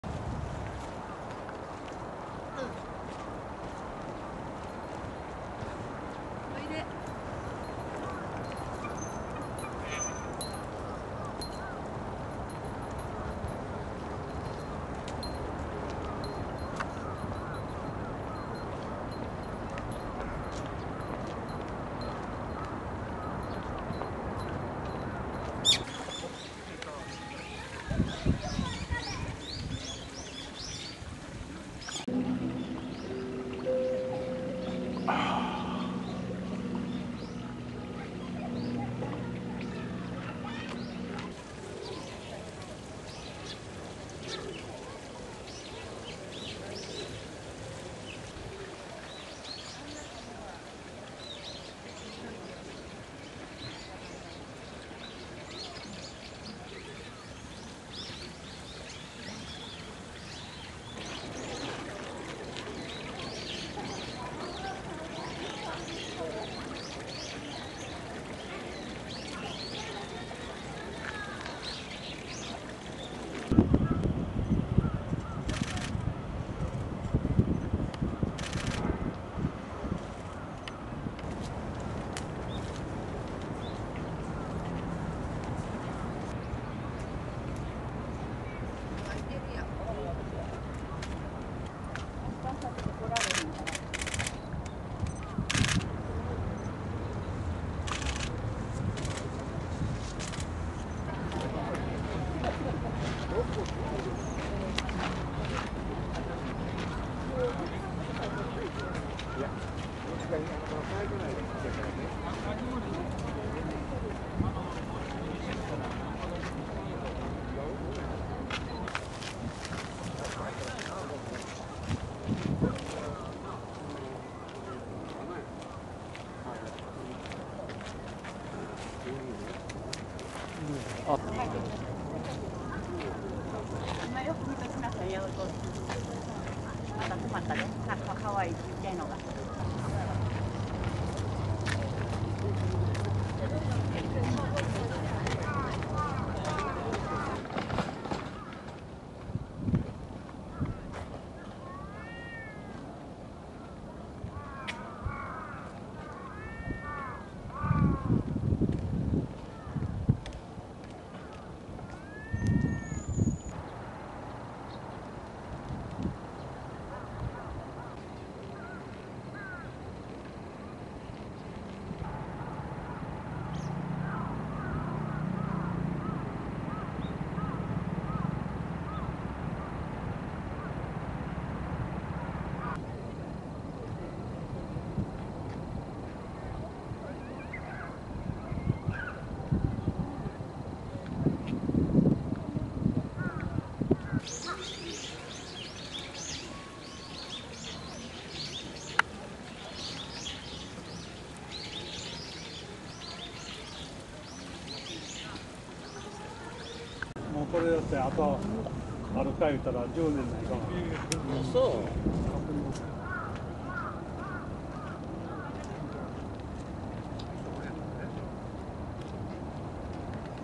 Birds of Osaka
Some of the birds of Osaka, Japan. Highlights are the Japanese Waxwing, Northern Shoveler and Japanese Cormorant.